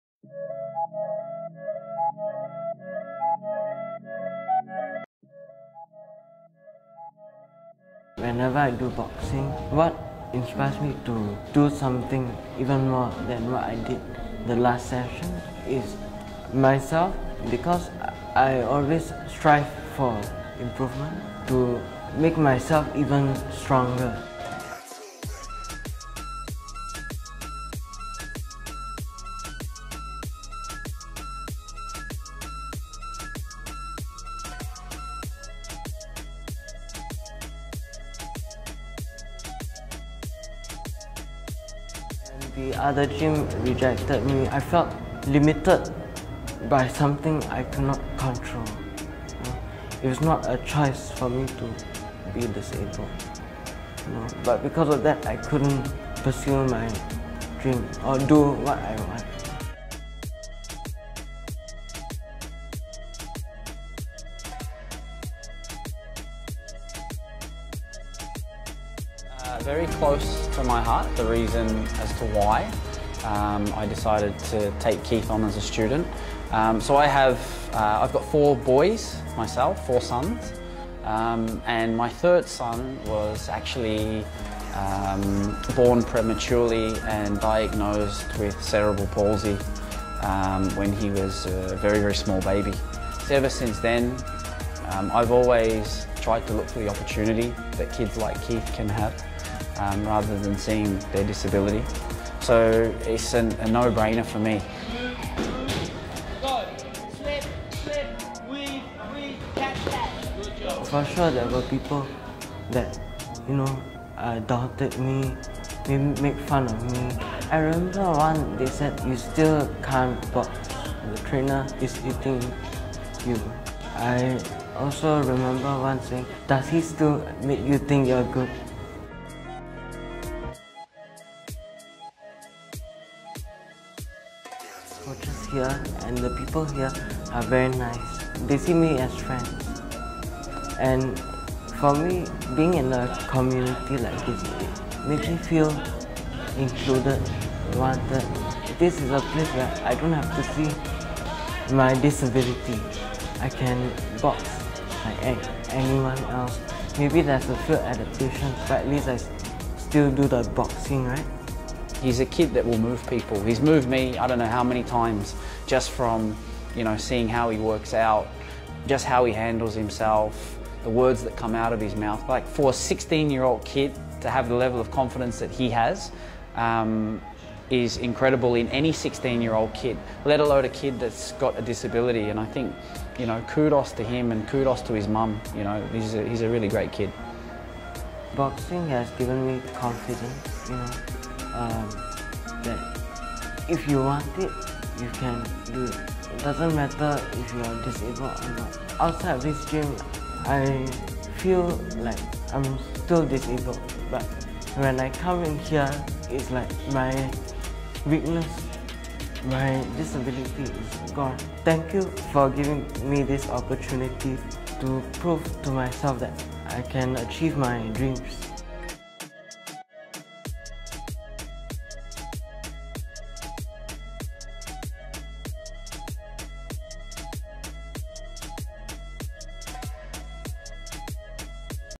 ROOMAIF | Spartans | Boxing Fight Training | Boxing was once an impossible Dream for Keith.
Taking up boxing was once an impossible dream for 16-year-old Keith. Then, he found someone (Russell Harrison) who believed in him.

Related to Boxing Fight, Boxing Events, Combat Sports.